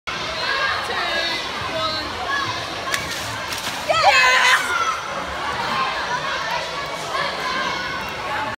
Swim cap slap, Wey Valley SC
Jake, Finley and Sam from Wey Valley Swimming club complete the Swim Cap Slap challenge, and double it!